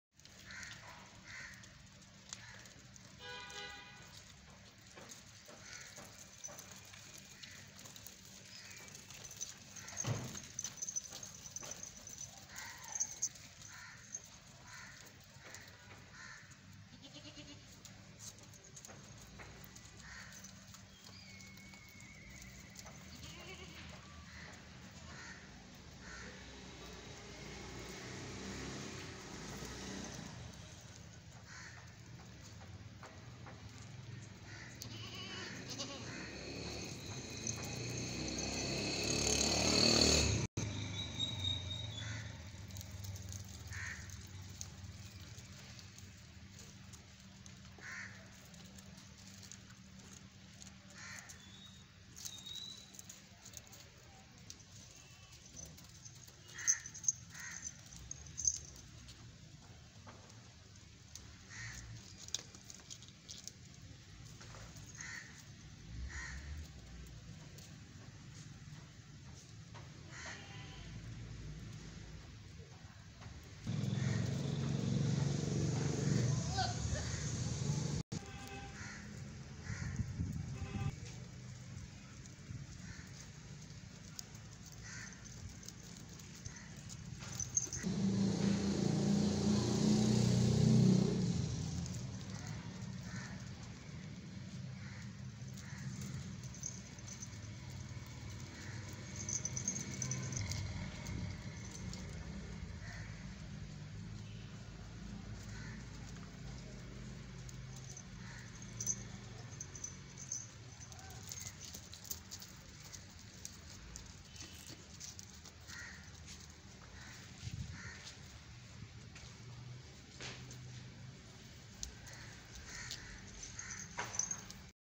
Modern goat farming |Beautiful Goats|Boer goat in Tharparkar
My land thar becomes heaven after rain
Beautiful goats are going for the prey to the pasture.